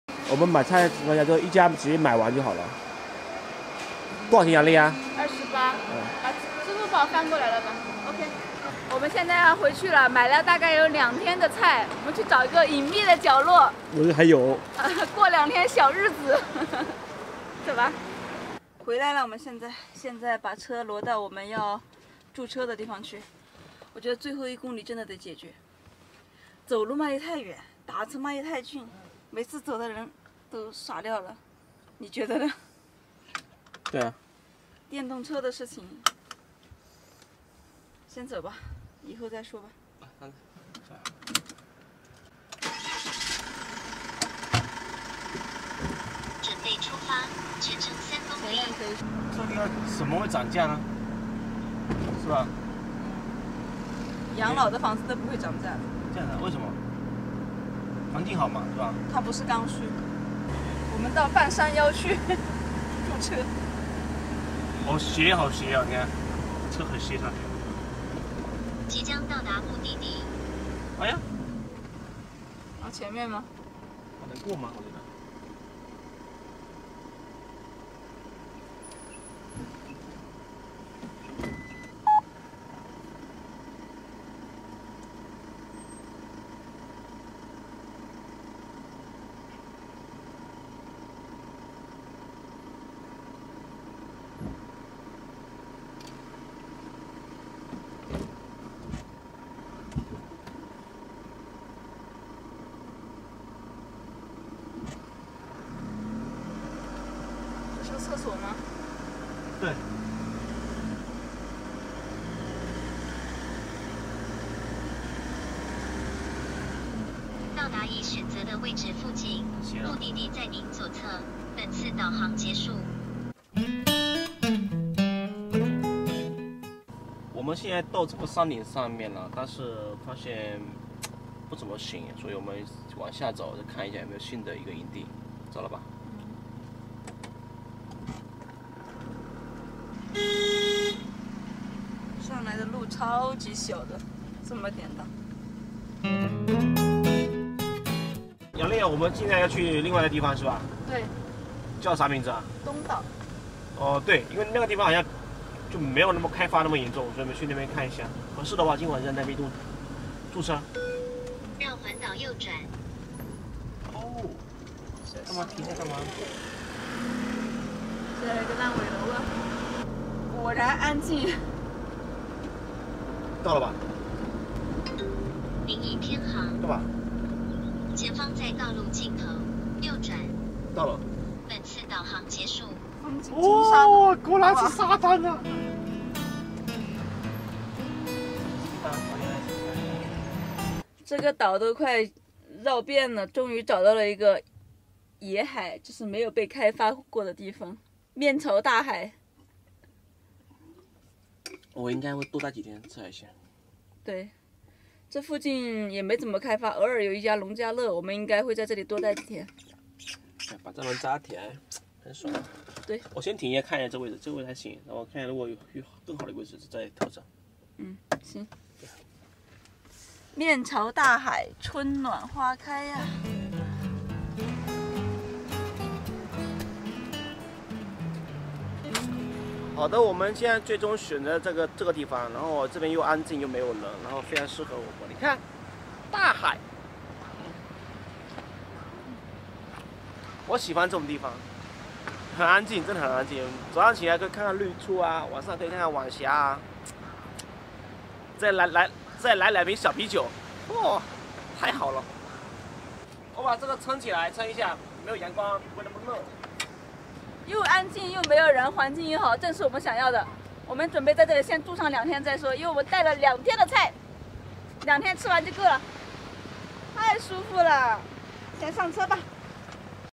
房车准备更换营地，带上点吃的准备闭关两天，找到一个绝佳驻车地第299期
房车准备更换营地，带上点吃的准备闭关两天，找到一个绝佳驻车地